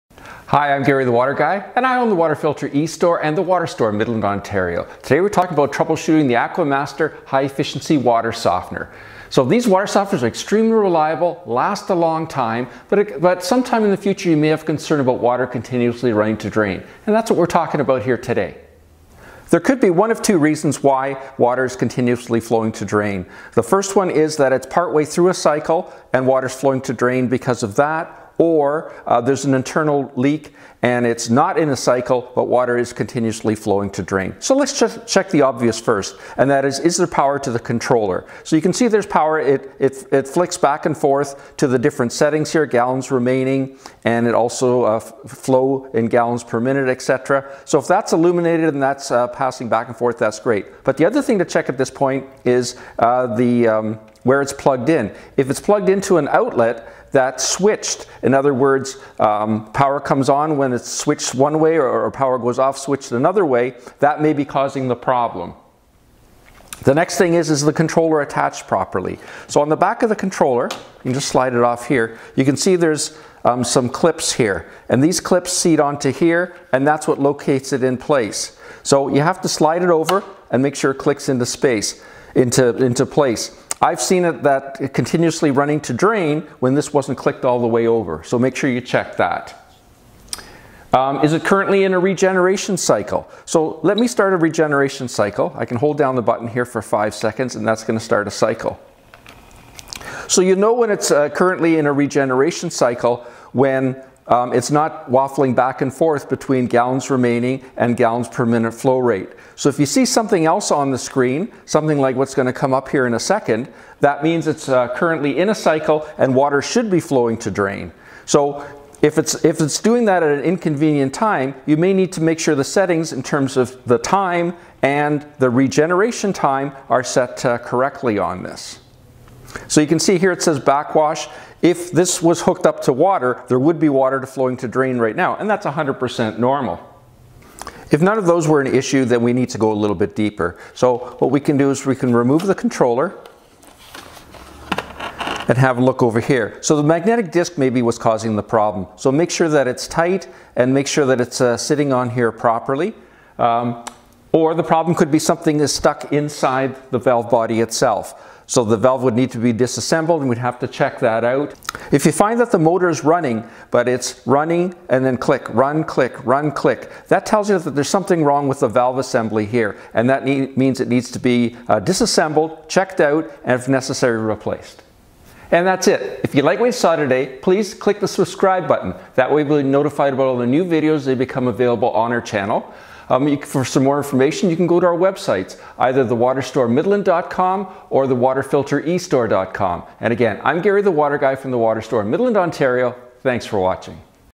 Water Softener Trouble Shooting Water Keeps Flowing to Drain - Aquamaster or Water Boss
or our full service store in Midland, Ontario at https//
 Although these Aquamaster High Effiency water softeners are very reliable, one day there may be a concern when you notice that water keeps flowing to the drain.
I always recommend you check the obvious first!
1)  Is the water softener currently in a regeneration cycle?  If the current time is incorrect the water softener may think that now is the time it regenerates, check the time displayed on the controller and if incorrect, correct it.
2)  Is the controller attached properly? Make sure the controller is pushed all the way onto the drive end cap and snapped into place.
3)  Is the water softener plugged in?  Check that the transformer is plugged into the controller.   Make sure there is electric power at the outlet. Make sure the outlet is not controlled by a switch and is occasionally switched off.
4)  Is the transformer defective?  Test with volt meter for 12 VAC at control. If less than 10 VAC or greater than 14 VAC, replace the transformer
5)   Defective magnet disk?  Replace magnet disk with a new one.
6)  Foreign object in valve body?  Remove foreign object(s) from the valve body
7)  Broken valve assembly? Motor running?  Repair or replace the drive end cap

Check out our online store at TheWaterFilterEstore.com or our full service store in Midland, Ontario at TheWaterStoreMidland.com

Please comment or ask questions below.